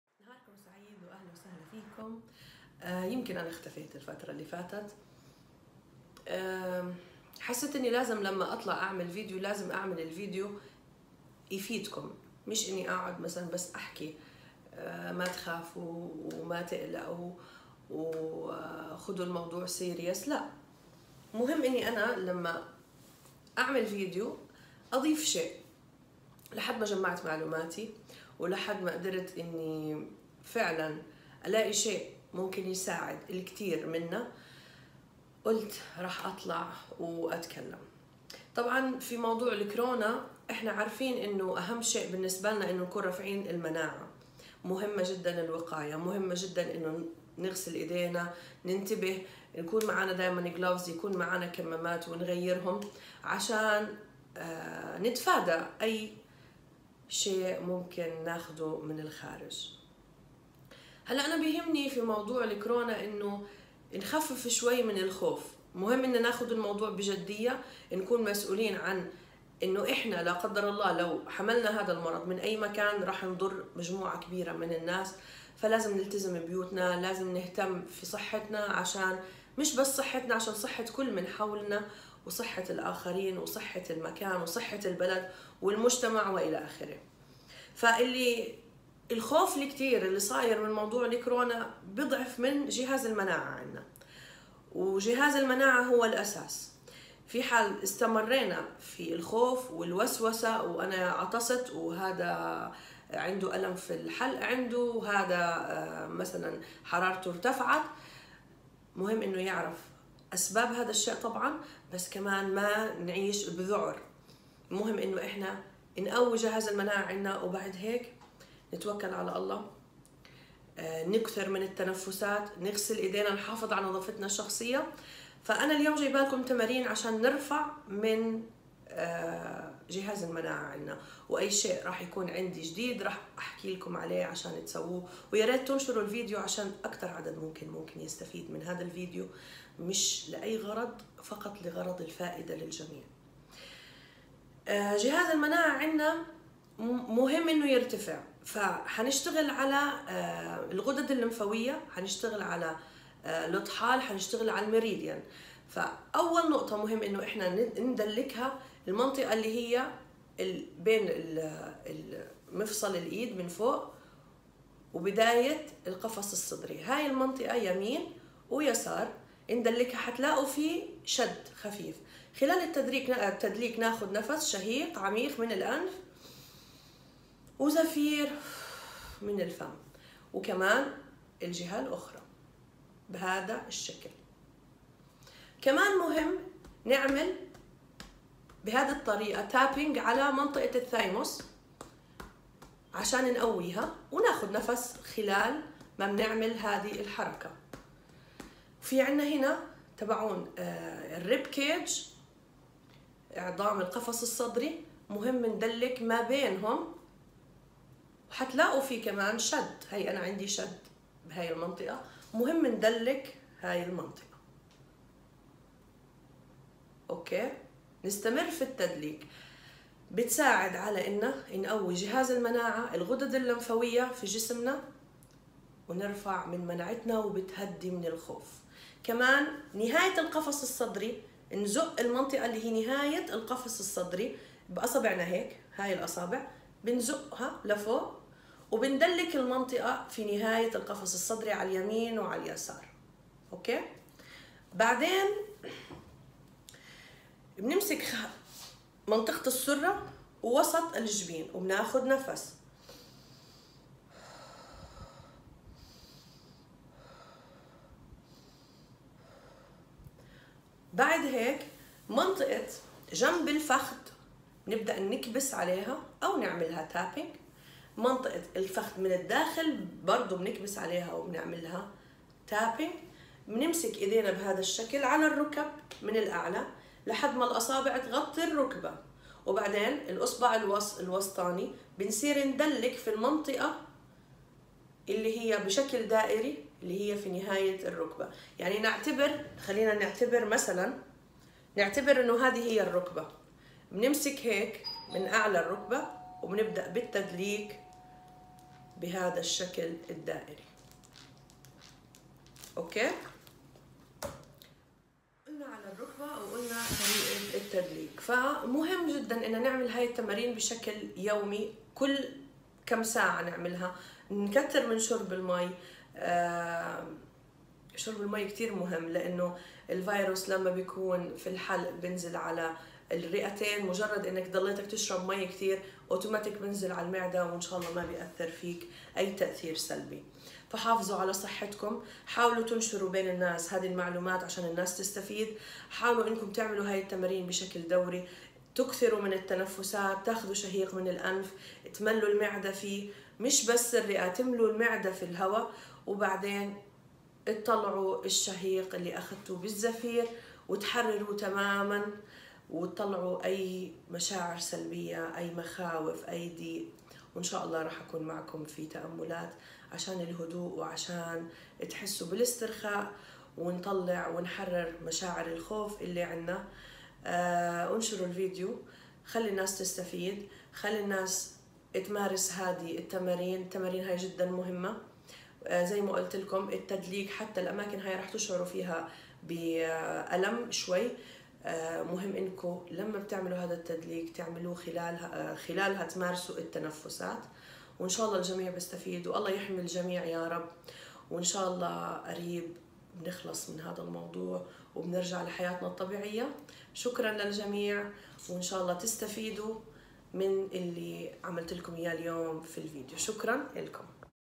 فايروس كورونا (تمارين لرفع مناعه الجسم )
يرجى ممارسه التمارين اكثر من مره في اليوم ، بالاضافه للتنفس والاكثار من شرب الماء والله يحفظ الجميع